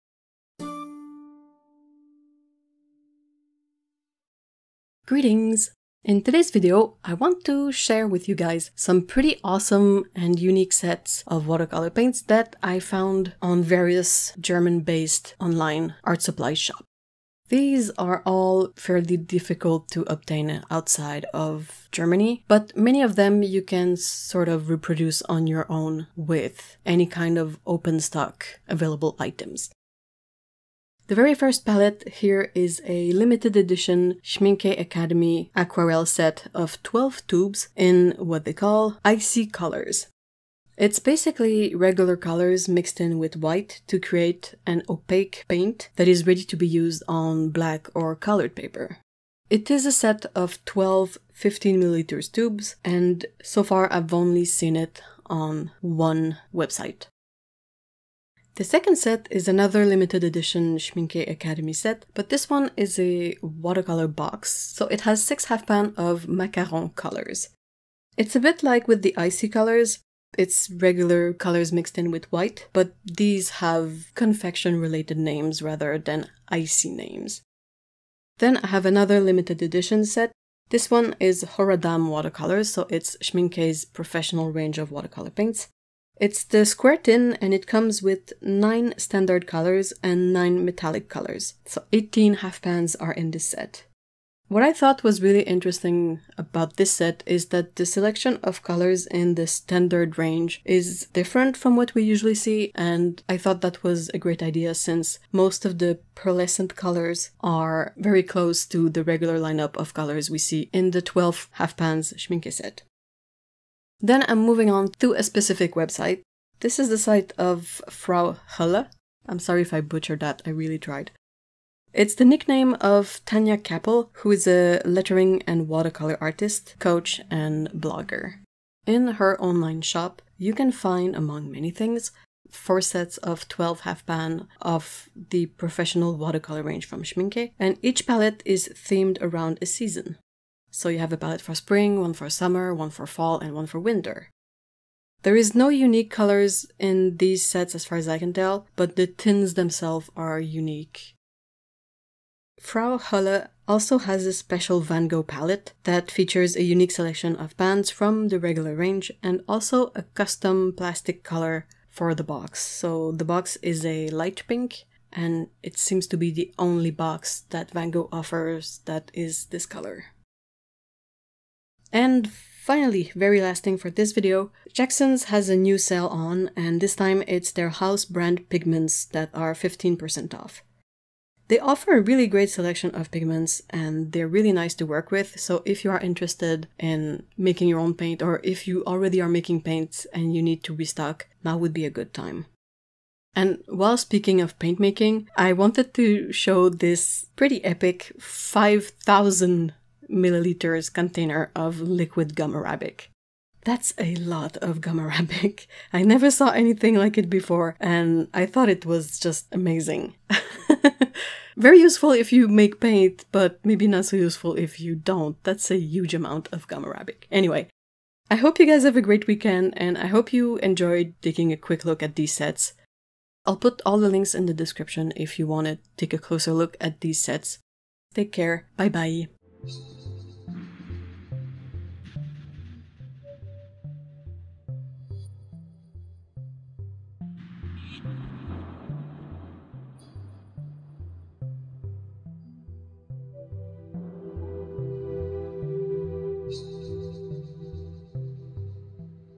Watercolor News🎨 - Special Watercolor Sets! - Schmincke, Van Gogh
Schmincke Horadam set of 18 half-pans (9 standard, 9 pearl metallics)
Thanks, Lukas, for letting me know about the Macaron and the mixed set! :)

**Linus is my cat "nephew" ♥